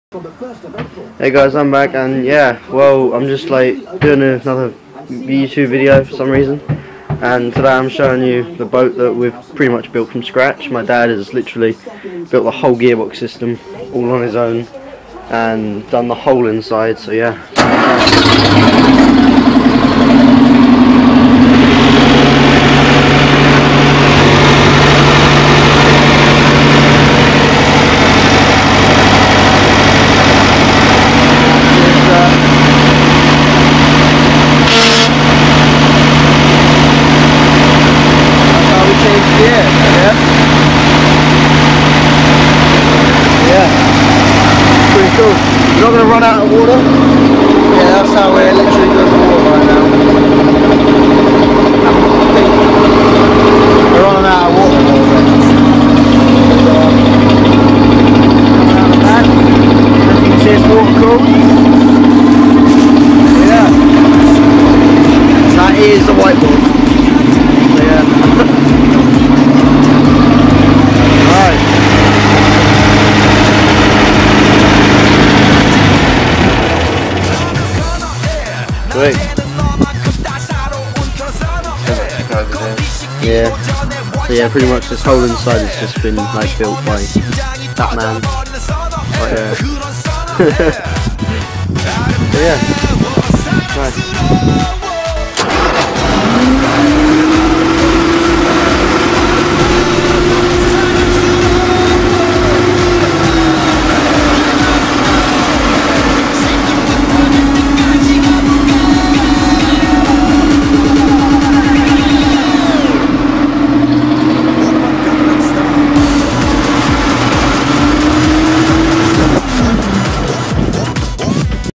Morice navigator boat
My dad built this boat from a shell he also built the gear box on his own